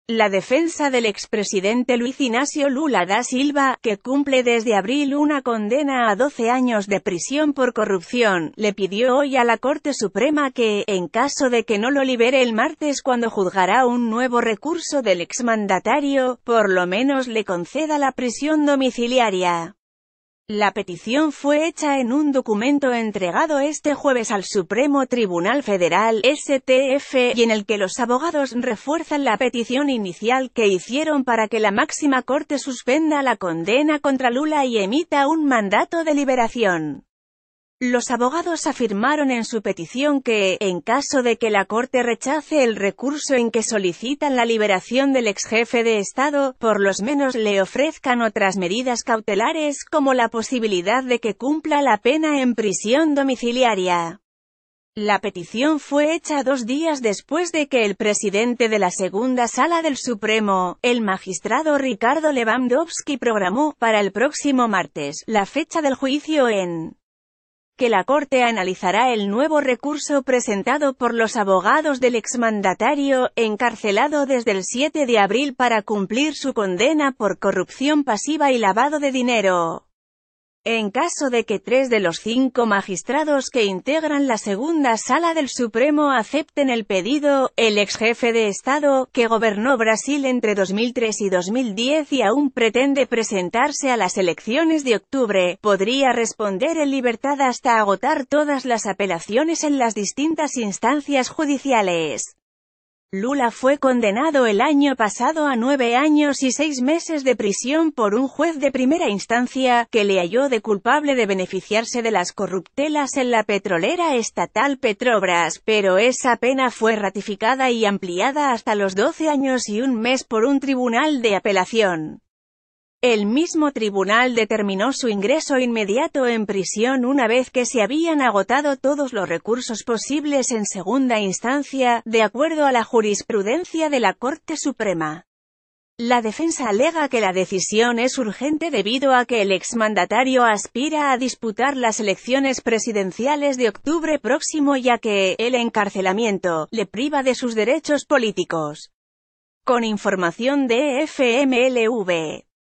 Piden prisión domiciliaria para Lula da Silva
La defensa del expresidente Luiz Inácio Lula da Silva, que cumple desde abril una condena a 12 años de prisión por corrupción, le pidió hoy a la Corte Suprema que, en caso de que no lo libere el martes cuando juzgará un nuevo recurso del exmandatario, por lo menos le conceda la prisión domiciliaria.